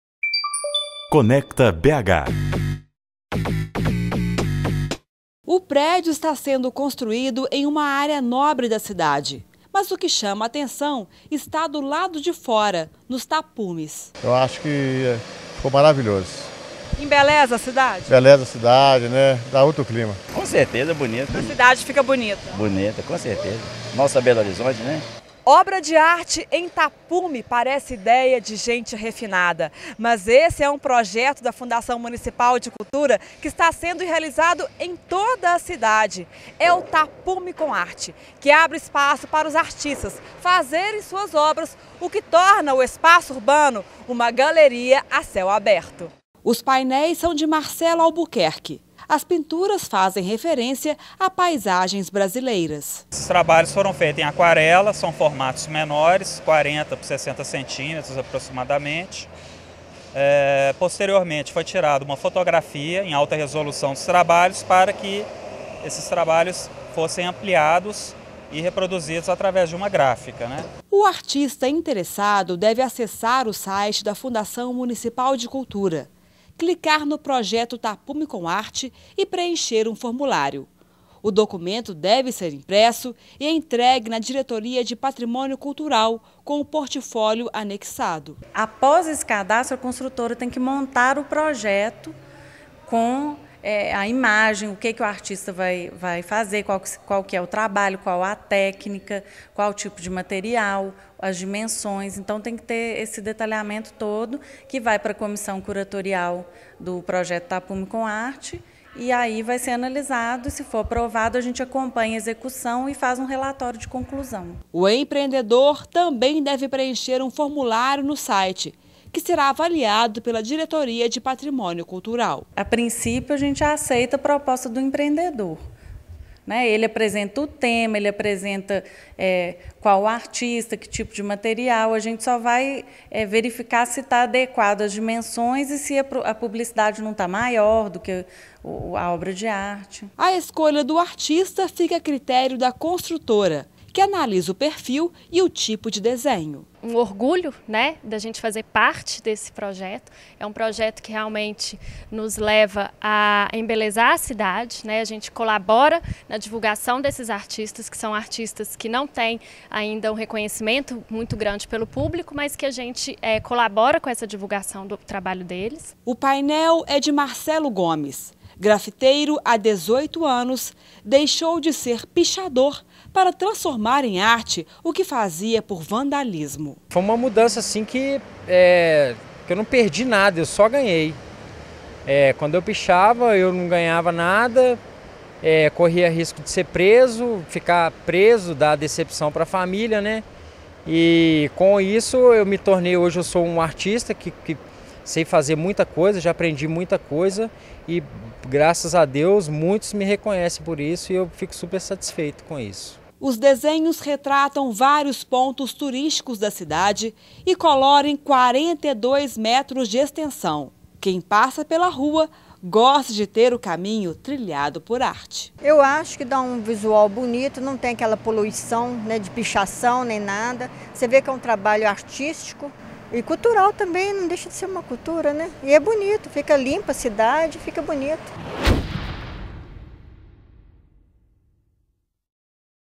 EPO I Matéria Conecta BH - Projeto tapume com arte
Projeto da Fundação Municipal de Cultura, o Tapume com Arte contribui para divulgar trabalhos de artistas e a embelezar a cidade. A EPO está participando do projeto em quatro tapumes de suas obras. Confira na reportagem.